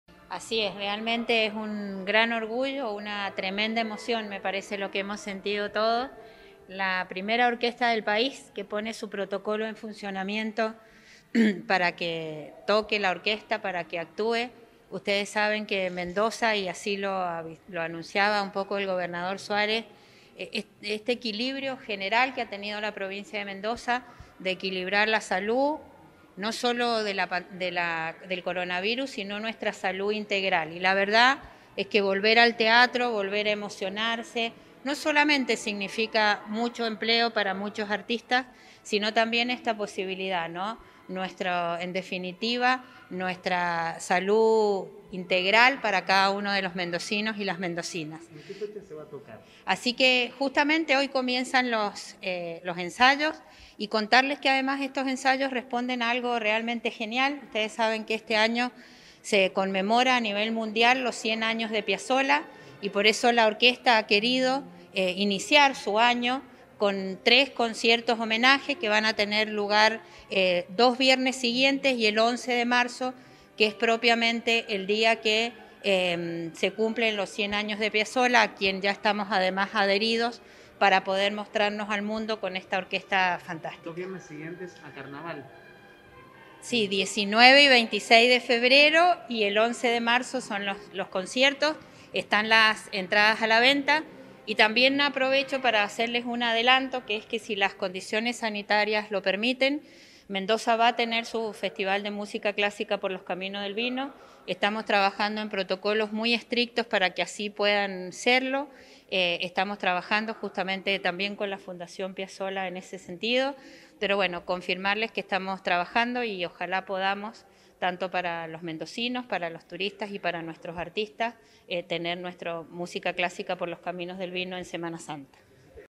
La Orquesta Filarmónica de Mendoza será la primera del país en dar un show presencial
Mariana Juri, Ministra de Turismo y Cultura Mendoza